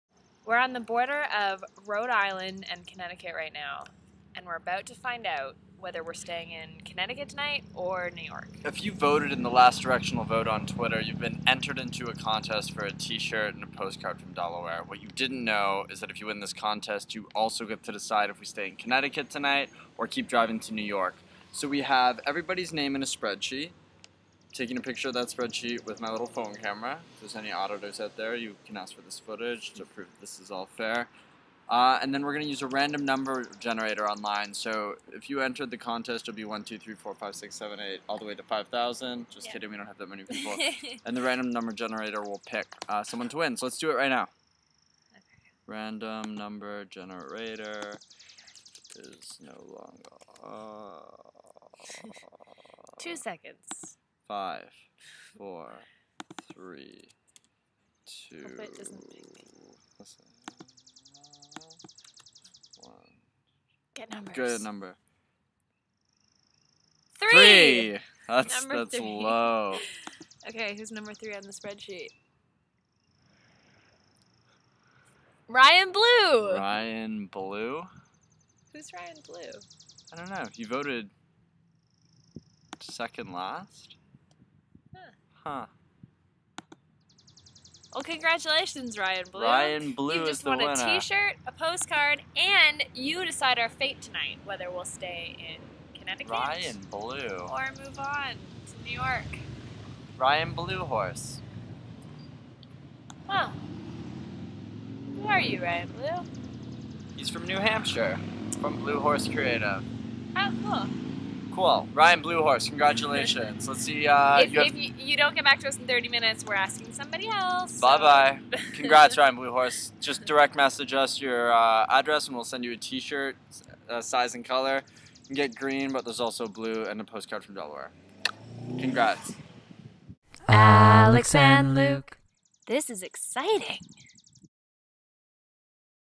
Twitter Vote Contest Winner!
We are letting a random twitter follower decide where we sleep tonight!